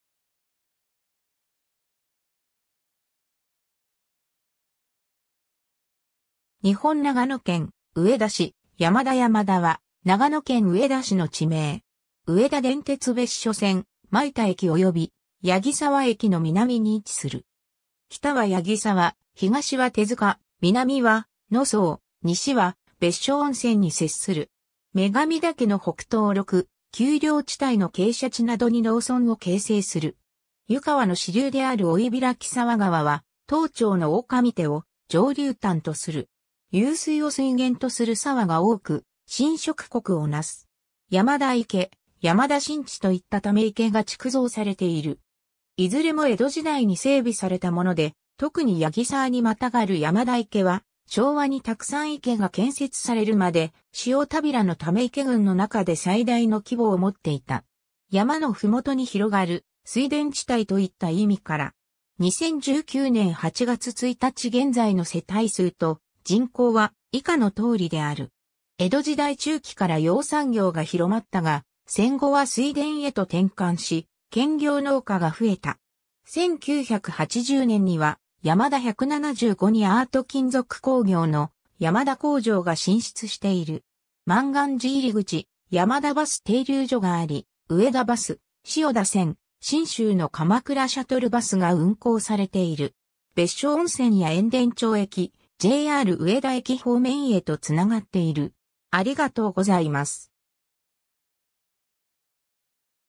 山田 (上田市)
上田市の町・字
日本 」 長野県 」 上田市 」 山田 山田（やまだ）は、長野県上田市の地名（町丁）。
 上田電鉄別所線・舞田駅および八木沢駅の南に位置する。
北は八木沢、東は手塚、南は野倉、西は別所温泉に接する。
女神岳の北東麓、丘陵地帯の傾斜地などに農村を形成する。
 湯川の支流である追開沢川（おかいざわがわ）は、当町の大上手を上流端とする。
湧水を水源とする沢が多く、侵食谷を成す。
 山田池・山田新池といったため池が築造されている。
いずれも江戸時代に整備されたもので、特に八木沢にまたがる山田池は、昭和に沢山池が建設されるまで、塩田平のため池群の中で最大の規模を持っていた。
 山（女神岳）のふもとに広がる水田地帯といった意味から。
 2019年（令和元年）8月1日現在の世帯数と人口は以下の通りである。
 江戸時代中期から養蚕業が広まったが、戦後は水田へと転換し、兼業農家が増えた。
 1980年（昭和55年）には山田175にアート金属工業の山田工場が進出している。
 「満願寺入口」・「山田」バス停留所があり、上田バス「塩田線」・「信州の鎌倉シャトルバス」が運行されている。
別所温泉や塩田町駅、JR上田駅方面へとつながっている。